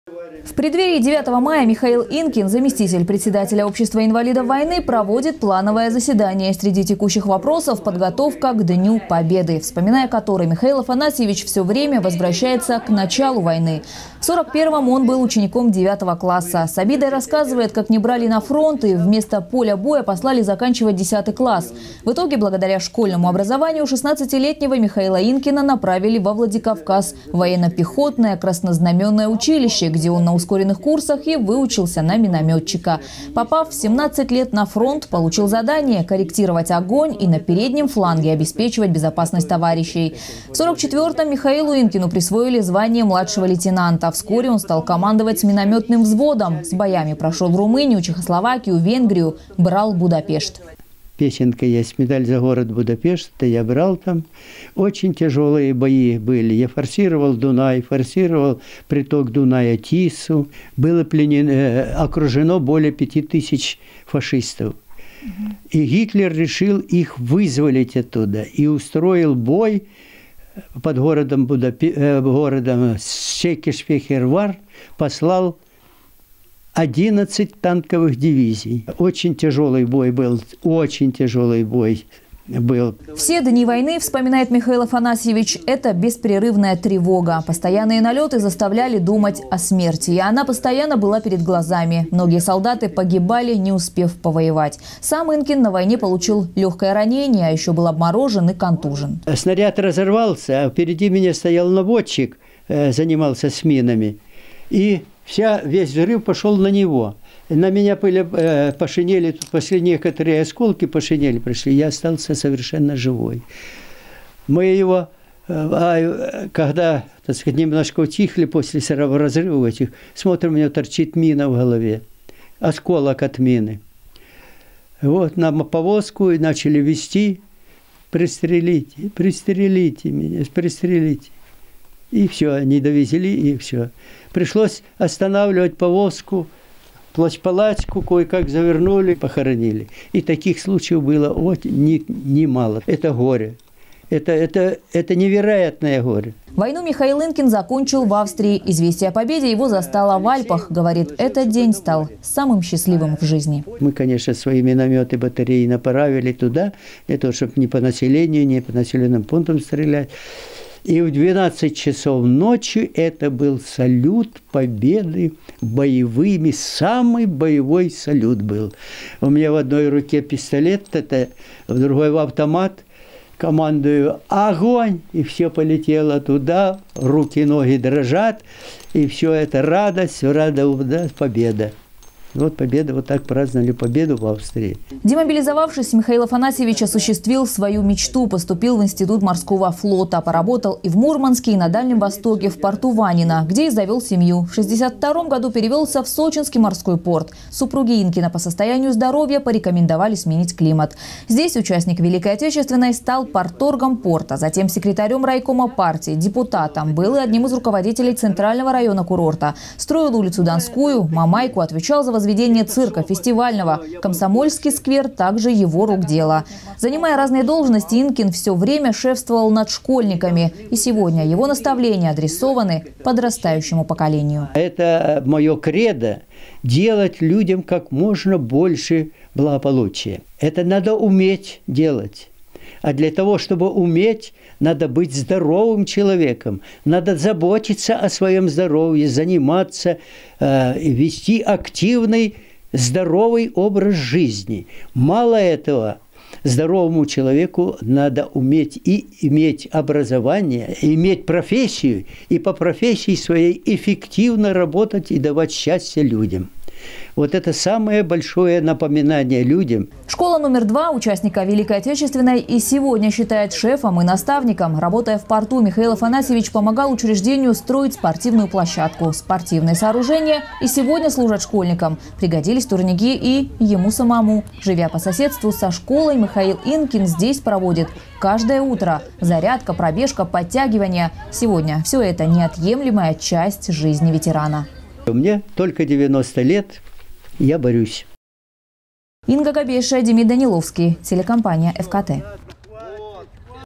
Портрет Ветерана: Михаил Инкин. Новости. Сочи. Эфкате.
Война на переднем фланге. Сочинец Михаил Инкин всю Великую Отечественную прошёл минометчиком. Сегодня ветерану 90 и он продолжает бороться за жизнь. Михаил Инкин занимается общественной деятельностью, ведёт здоровый образ жизни и каждый день проводит с пользой. В жизни как и на войне нужно быть эффективным, подчёркивает победитель.

В преддверии 9 мая Михаил Инкин, заместитель председателя общества инвалидов войны проводит плановое заседание. Среди текущих вопросов — подготовка ко Дню Победы. Вспоминая который Михаил Афанасьевич все время возвращается к началу войны.

В 41-ом он был учеником 9-го класса. С обидой рассказывает как не брали на фронт. И вместо поля боя послали заканчивать 10 класс. В итоге благодаря школьному образованию 16-летнего Михаила Инкина направили во Владикавказ, в военно-пехотное Краснознаменное училище, где он на ускоренных курсах и выучился на минометчика. Попав в 17 лет на фронт получил задание — корректировать огонь и на переднем фланге обеспечивать безопасность товарищей. В 44-ом Михаилу Инкину присвоили звание младшего лейтенанта. Вскоре он стал командовать минометным взводом. С боями прошёл Румынию, Чехословакию, Венгрию. Брал Будапешт.

Сочи. Новости.
Телеканал Эфкате.